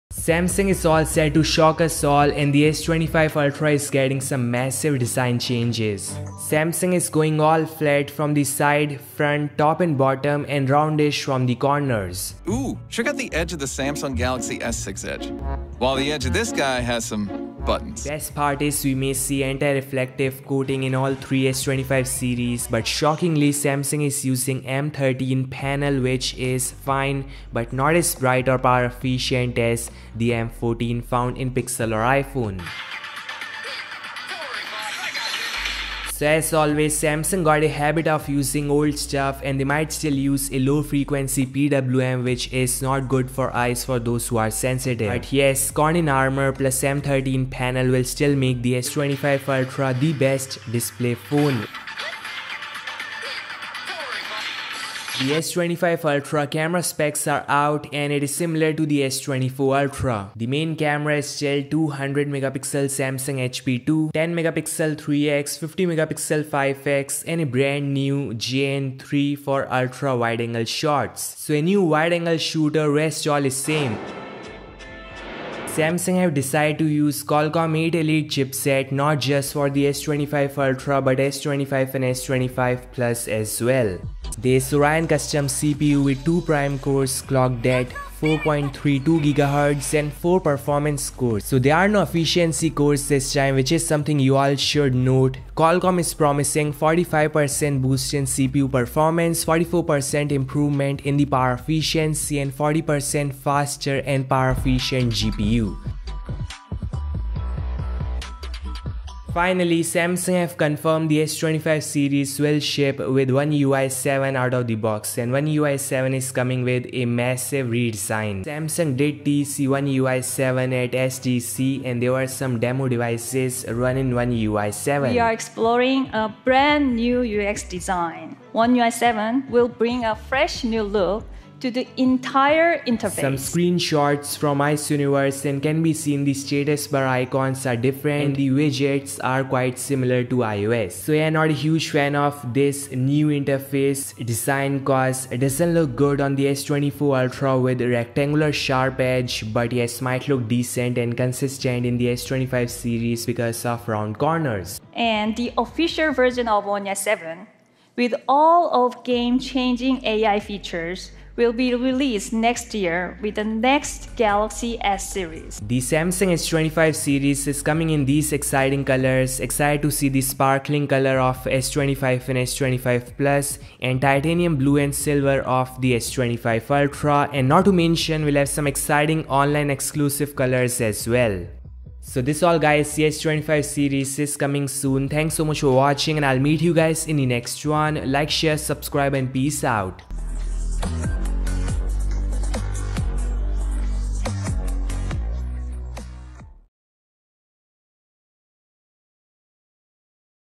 Samsung Galaxy S25 Ultra - GET READY!
Samsung most powerful Flagship is coming. This is the Samsung Galaxy S25 Ultra coming with Snapdragon 8 Elite Chipset (8 Gen 4) and 200MP Camera System, Bezel-less Display and Powerful Camera. Samsung S25 Ultra will have exciting titanium colors.